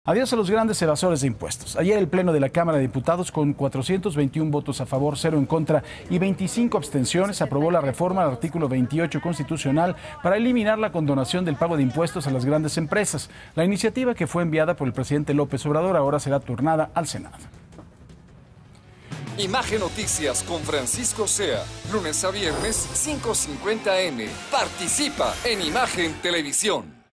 Diputados aprueban la reforma que elimina la condonación de impuestos | Noticias con Paco Zea
¡Adiós a los grandes evasores de impuestos!

Diputados aprueban la reforma que elimina la CondonaciónDeImpuestos.